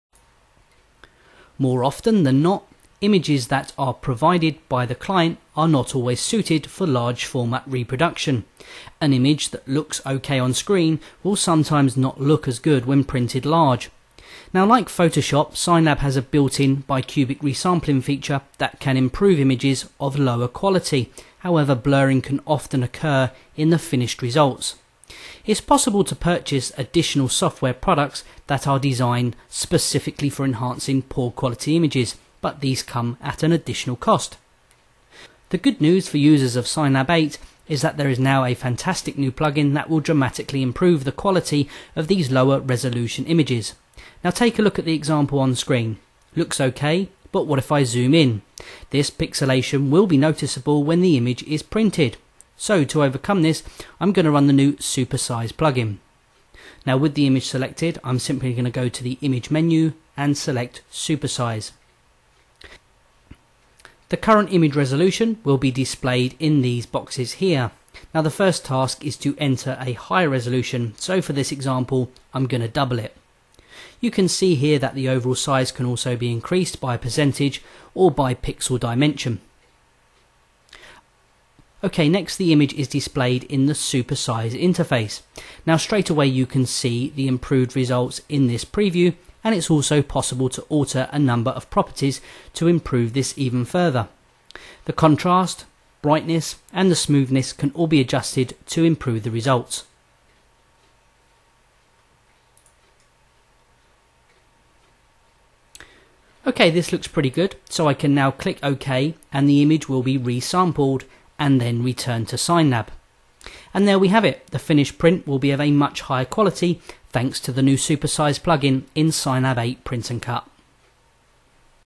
Super Size - CADlink SignLab Training Tutorial
Over 75 New Special Effects Added - All the special effects a designer needs are now accessible right from SignLab. We have integrated some of the industry's most popular filter plug-ins right into SignLab. No need to jump back and forth between design applications to get that "WOW" factor into your designs!

This instructional video takes you through a step-by-step guide to using some of these features of CADlink SignLab sign design and large format print software.